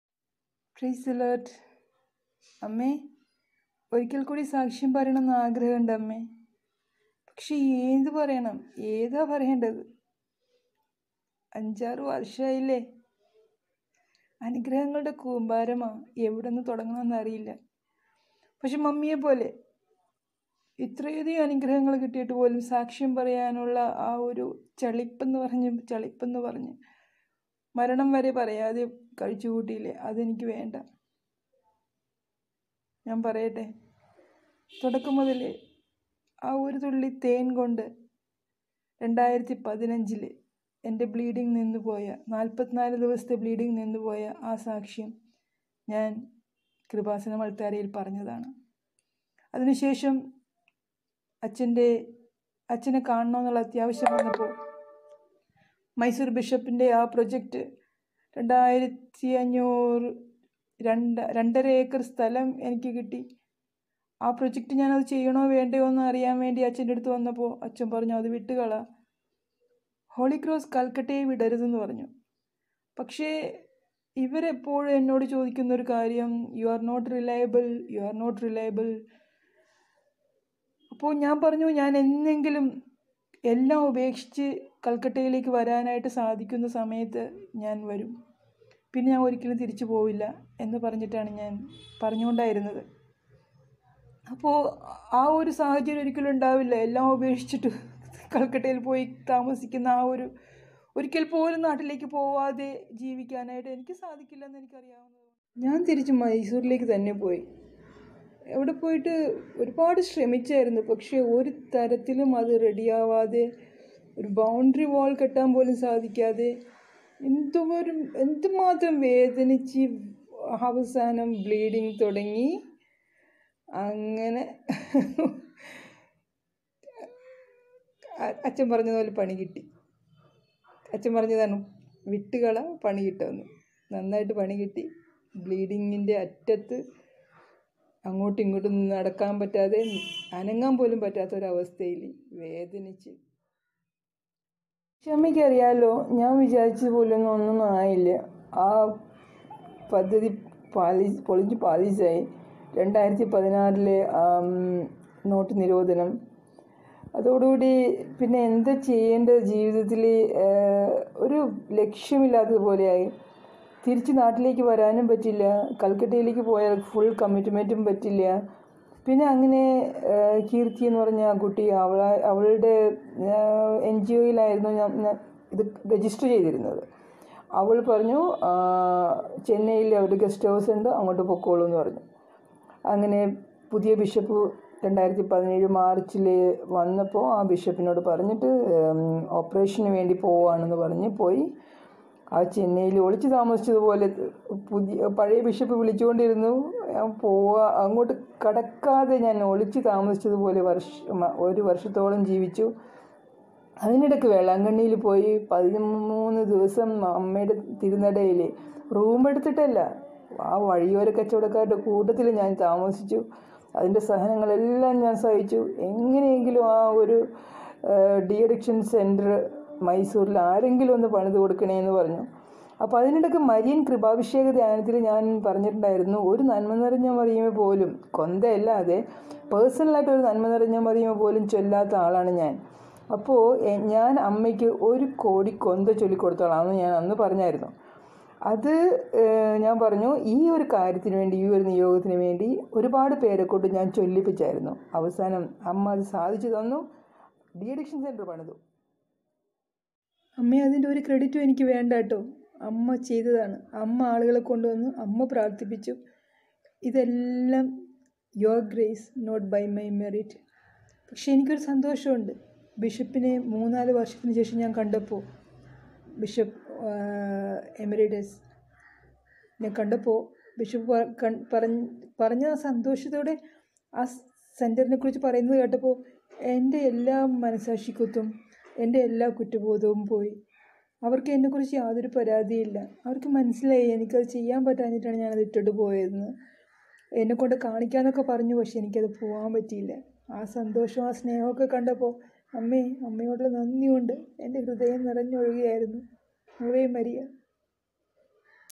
ഇതും Kreupasanam മാതാവിൻ്റെ അത്ഭുതം (സാക്ഷ്യം 2) see description box
ആദ്യത്തെ സാക്ഷ്യം ഇതാണ്

ഒറ്റത്തുള്ളി തേൻ കൊണ്ട് 44 ദിവസം നീണ്ടുനിന്ന ബ്ലീഡിംഗ് പിടിച്ചു കെട്ടിയത് പോലെ നിന്നുപോയി 😇 PLH2U